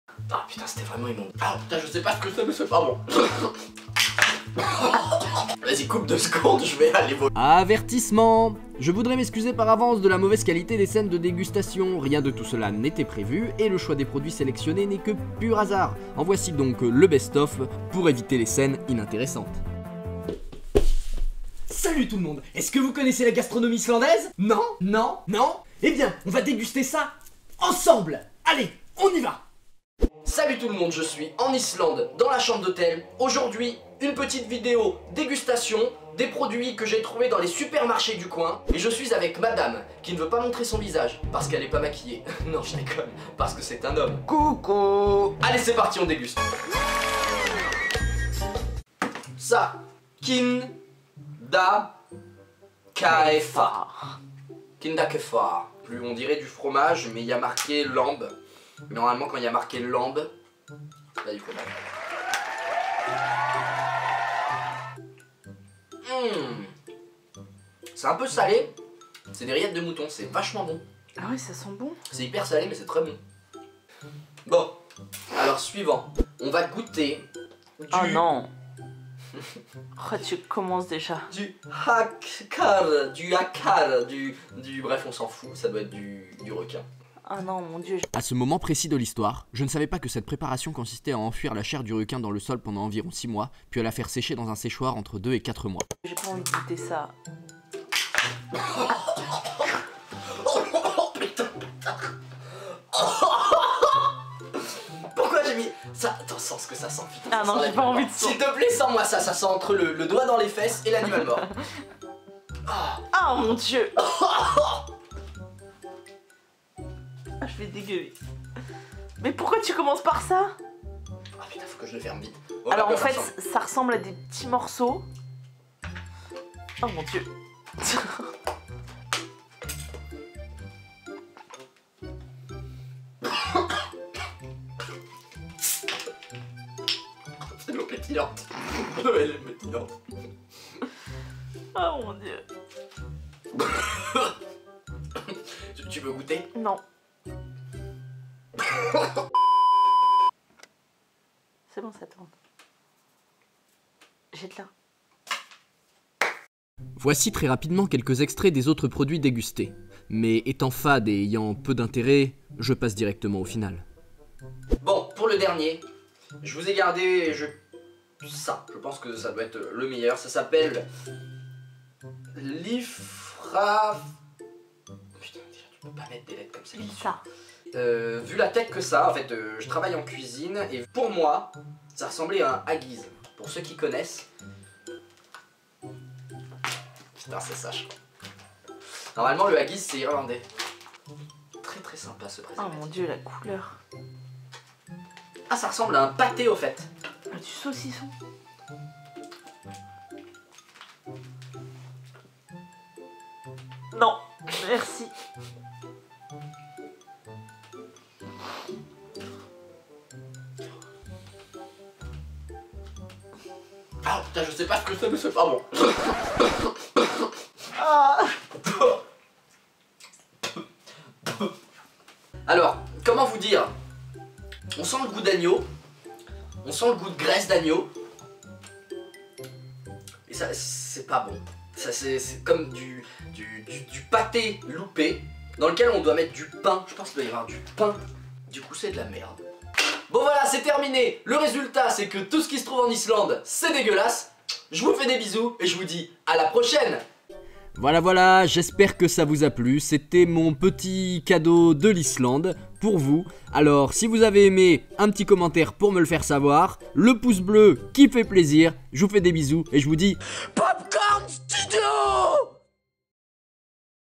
Le HAKARL, Requin Fermenté D'Islande #1 [ÉTRANGES DÉGUSTATIONS]
Le HAKARL, Requin Fermenté D'Islande [BONUS]

Salut tout le monde me voilà de retour d'ISLANDE et j'ai une surprise pour vous!
J'ai goûté les produit locaux et c'était pas ce qu'on pourrait appelé un régale...